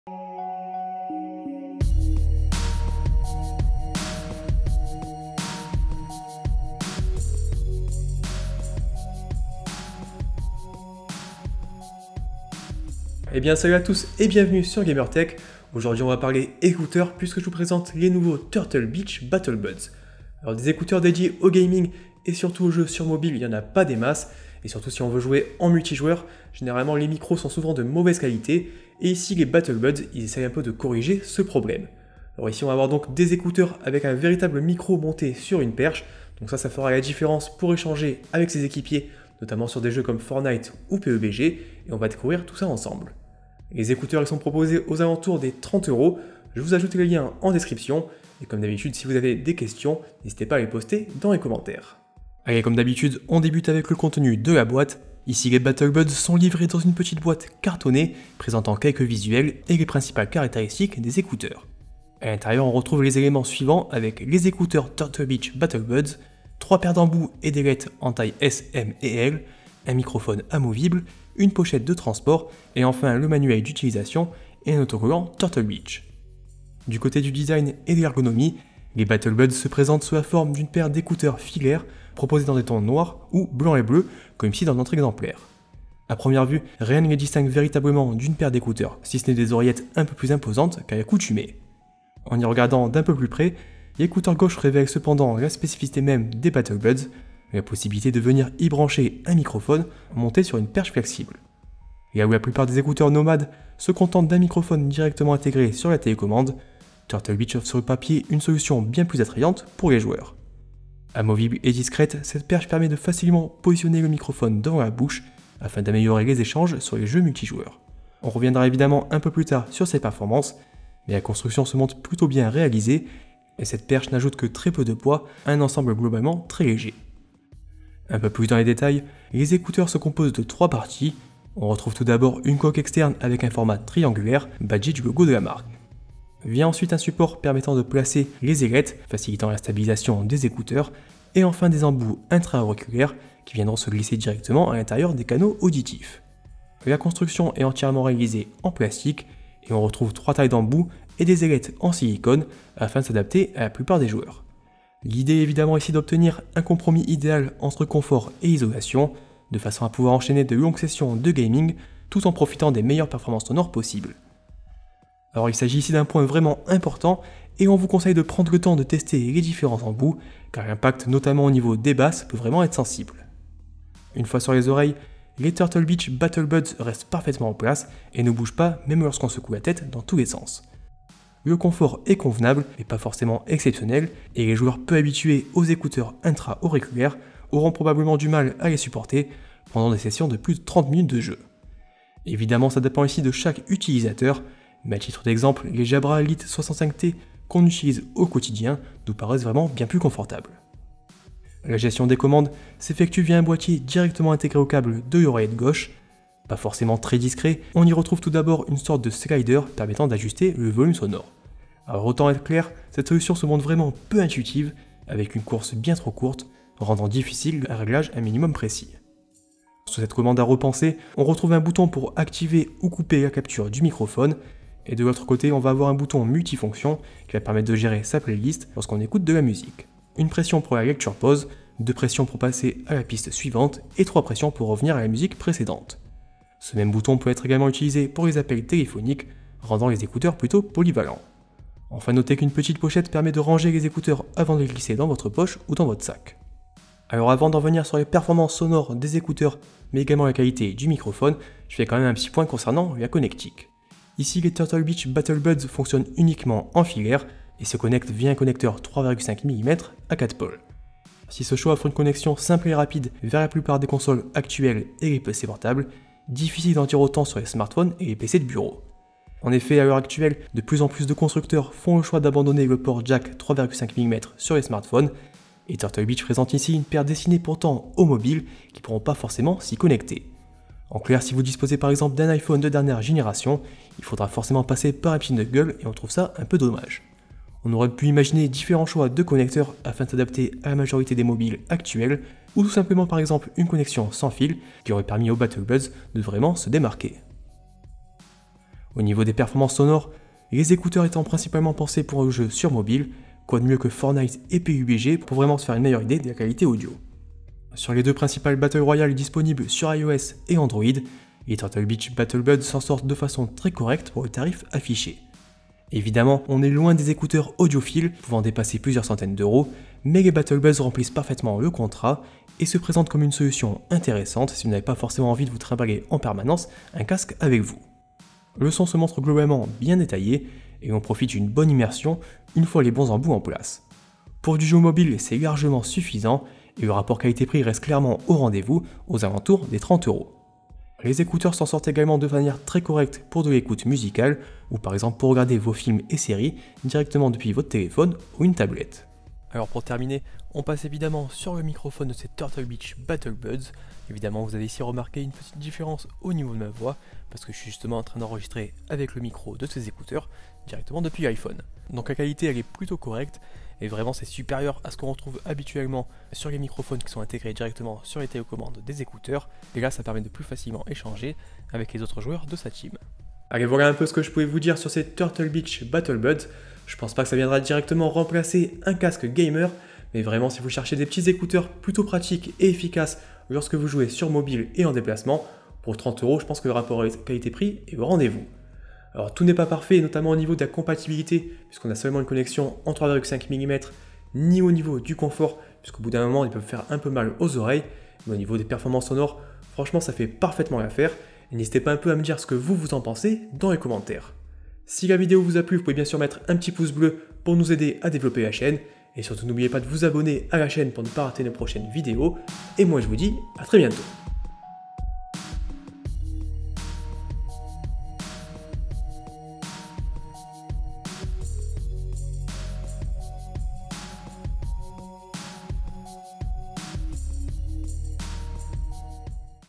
Turtle Beach Battle Buds | TEST | Des écouteurs pour gamer à moins de 30€
🎧 Présentation des écouteurs Turtle Beach Battle Buds !

Disponibles sous la barre des 30€, les écouteurs Turtle Beach Battle Buds visent principalement le secteur du jeu sur mobile. Pensés pour les adeptes des Battle Royal tels que Fortnite ou PUBG, ils permettent d'écouter les autres joueurs mais surtout de se faire entendre grâce à un véritable microphone monté sur une perche amovible.

Une solution intéressante pour les joueurs les plus nomades, n'ayant pas forcément envie de se trimbaler un gros casque gamer lorsqu'ils jouent sur leur smartphone ou tablette.

➡️ Panasonic G7
➡️ Micro Blue Yeti
➡️ Trépied Manfrotto
➡️ Rotule vidéo

GamerTech - Un Gamer bien équipé en vaut deux !